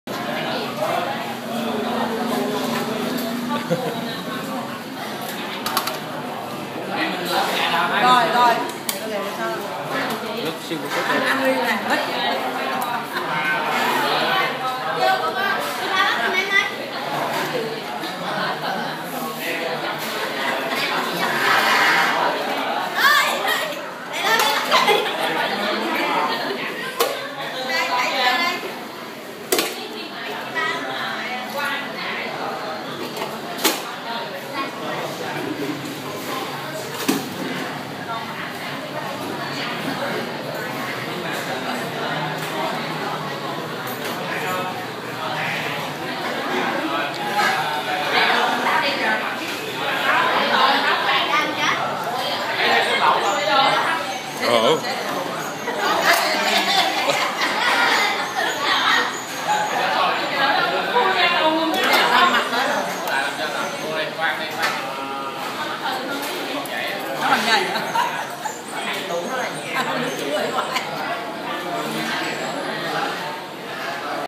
Lau ca keo hot pot
A way to cook fish they use in Vietnam